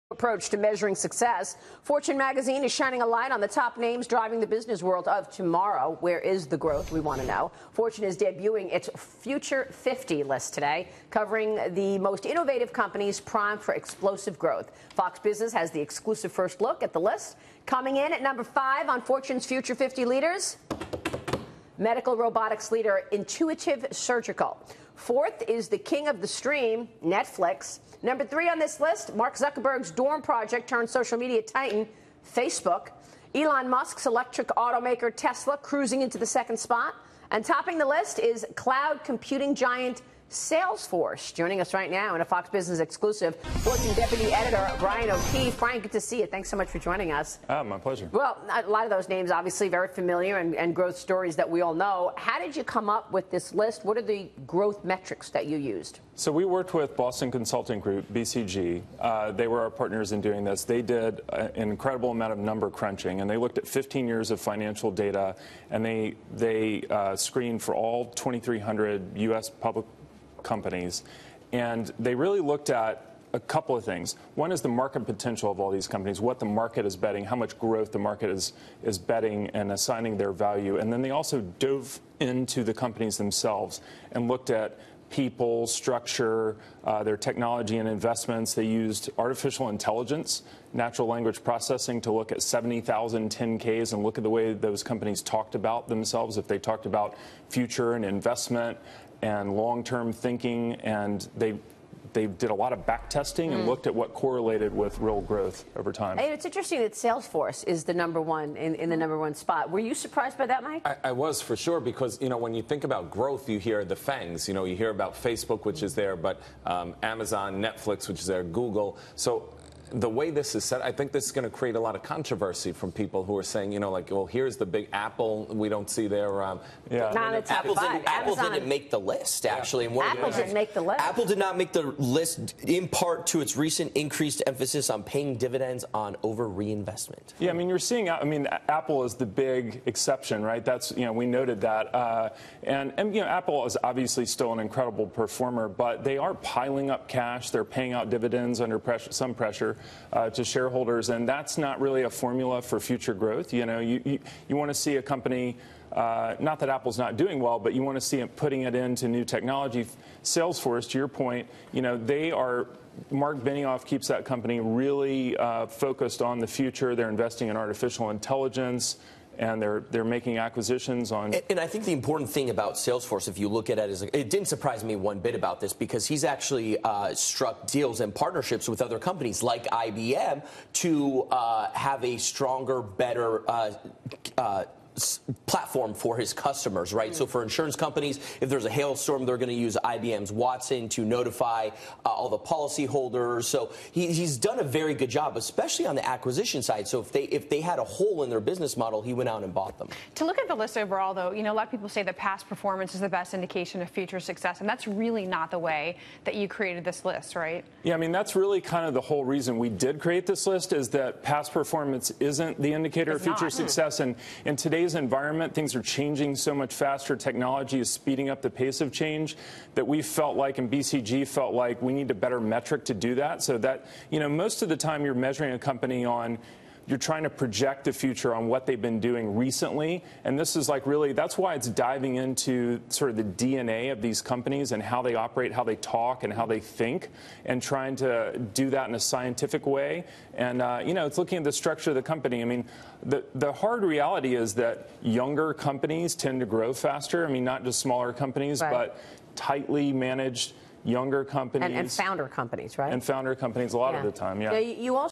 The top companies driving the business world of tomorrow
Fortune Deputy Editor Brian O'Keefe on the most innovative companies primed for explosive growth.